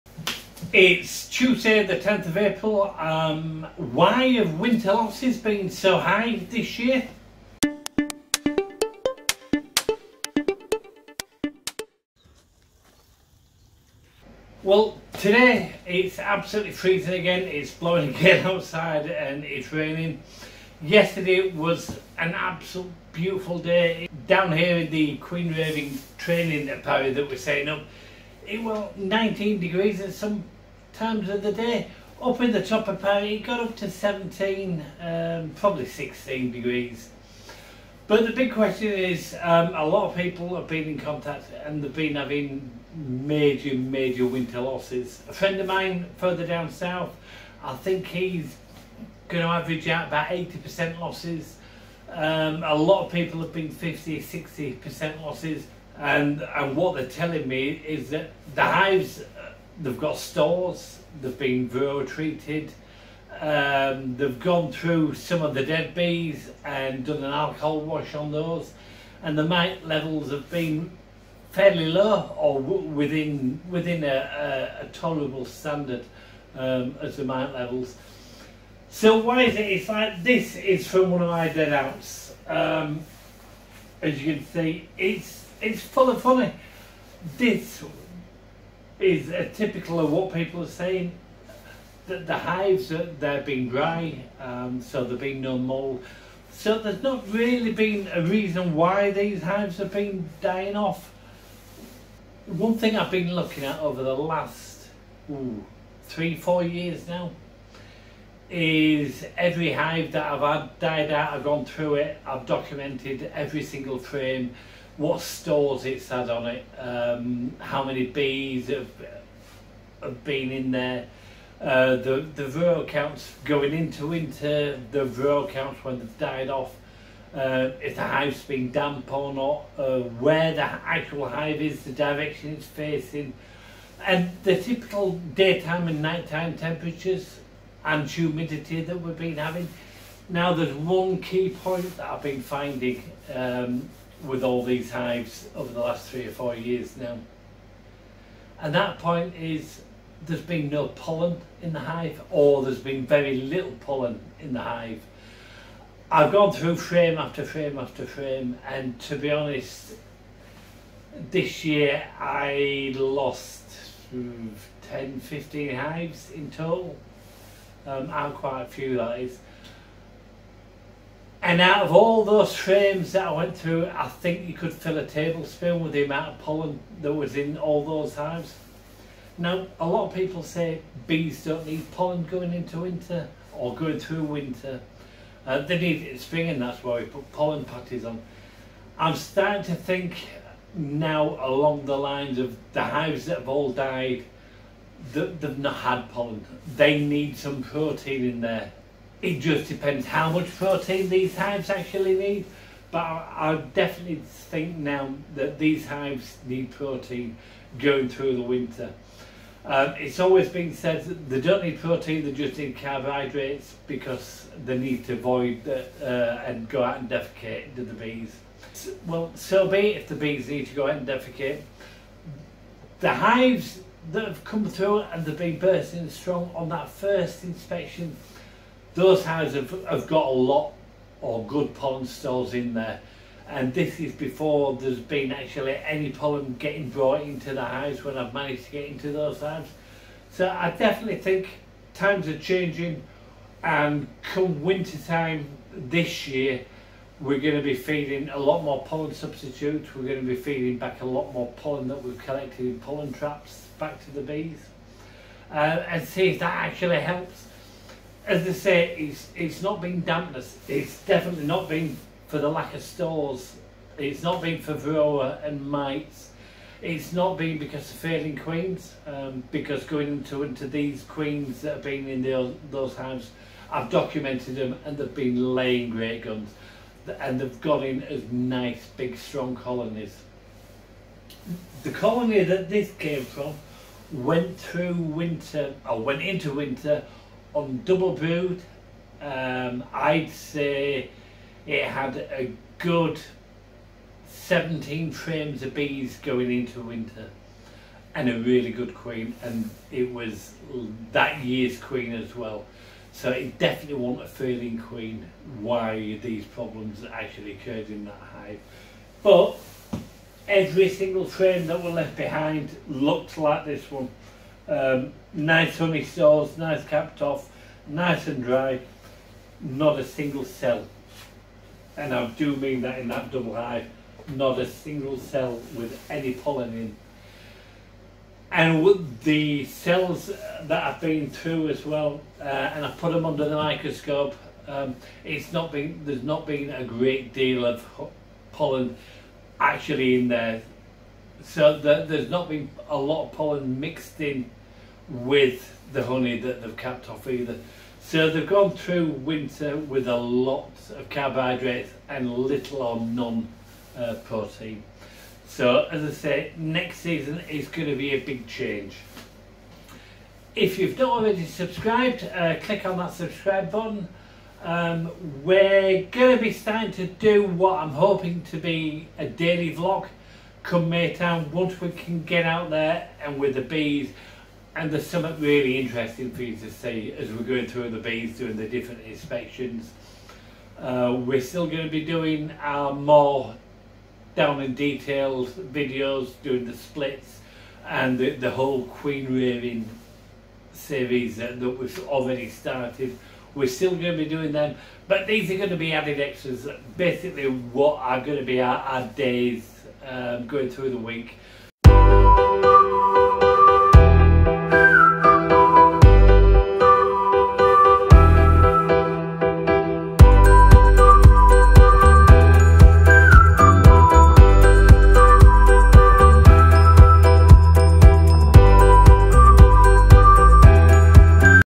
Honey Bee High losses Winter 2017 Spring 2018 Why?
Why are losses so high again not just the UK but in mainland Europe USA and Canada.
These are just my thoughts and not proven. With temperatures swinging from mild to bitter all winter long it could be that the fatter winter bees are just running out of steam or they're not getting to put the fat before going into winter.

This year I have started feeding the bees a mix of syrup with pollen sub mixed in as well as having the open feeders filled with Mannlake Ultra Bee pollen substitute.

Samsung S8
Neewer Tripods

 Feel Free To send me stuff I Love Coffee!!